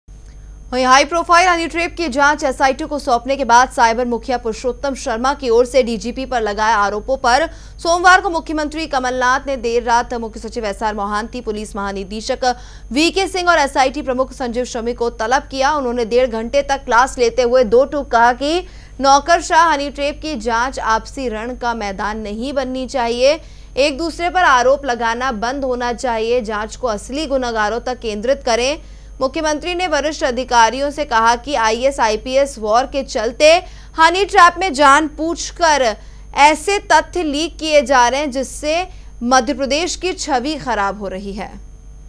MP GOVT.CM नौकरशाह हनीट्रैप की जाँच आपसी रण का मैदान नहीं बननी चाहिए
हाईप्रोफ़ाइल हनीट्रैप की जाँच SIT को सौंपने के बाद साइबर मुखिया पुरुषोत्तम शर्मा की ओर से DGP पर लगाए आरोपों पर सोमवार को मुख्यमंत्री कमला ने देर रात मुख्य सचिव एसआर मोहांती पुलिस महानिदेशक VK सिंह और SIT प्रमुख संजीव शमी को तलब किया उन्होंने डेढ़ घंटे तक क्लास लेते हुए 2 टूक कहा कि नौकरशाह हनीट्रैप की जाँच आपसी रण का मैदान नहीं बननी चाहिए... एक दूसरे पर आरोप लगाना बंद होना चाहिए जाँच को असली गुनाहगारों तक केंद्रित करें मुख्यमंत्री ने वरिष्ठ अधिकारियों से कहा कि IAS IPS वॉर के चलते हनीट्रैप में जानबूझकर ऐसे तथ्य लीक किए जा रहे जिससे मध्य प्रदेश की छवि ख़राब हो रही है...